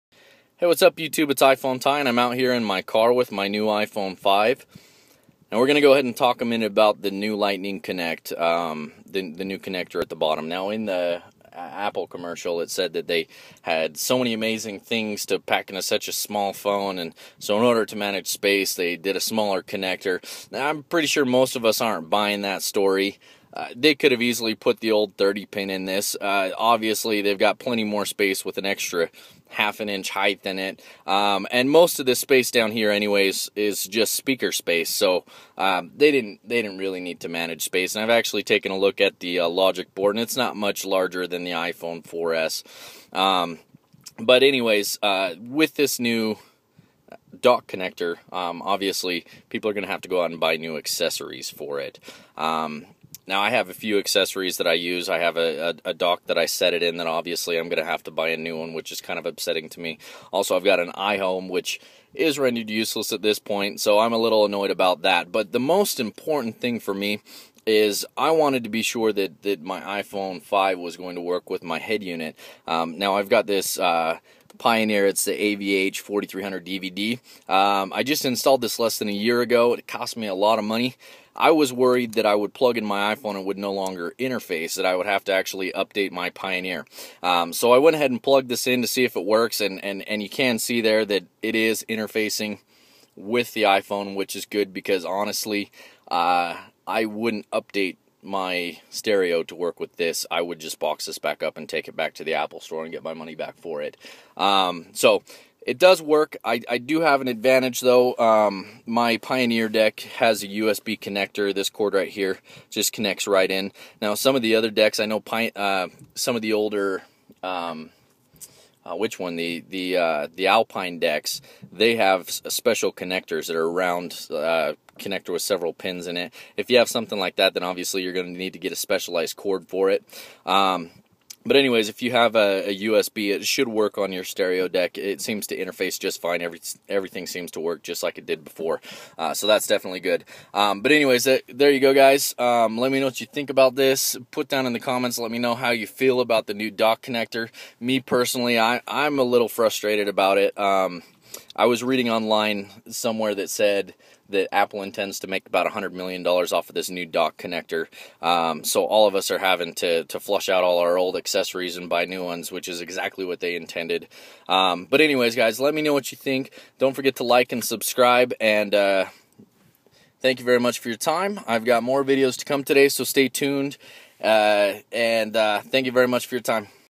iPhone 5 Lightning Connect Review/ Interfacing w/ Car Stereo
Will the iPhone 5 interface with your car stereo? I try my iPhone on my pioneer avh-p4300dvd head unit to see if it will interface.